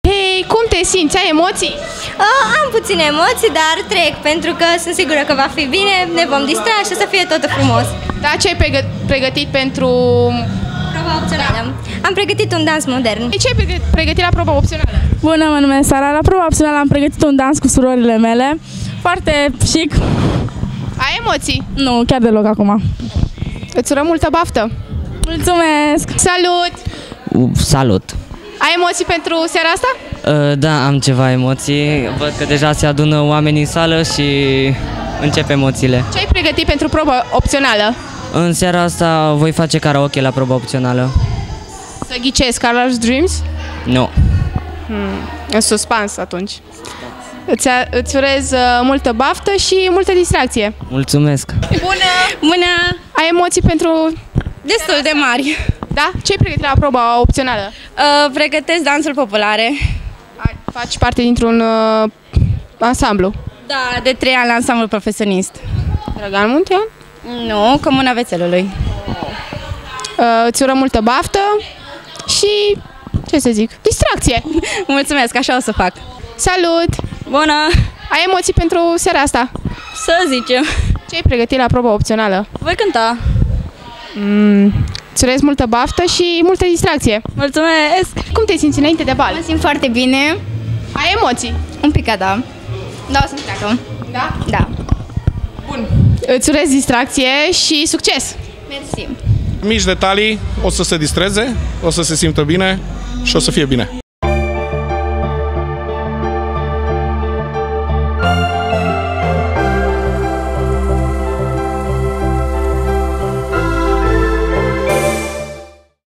Inainte de BALUL BOBOCILOR 2019
Interviuri Balul Bobocilor Liceul Grigore Moisil Deva 2019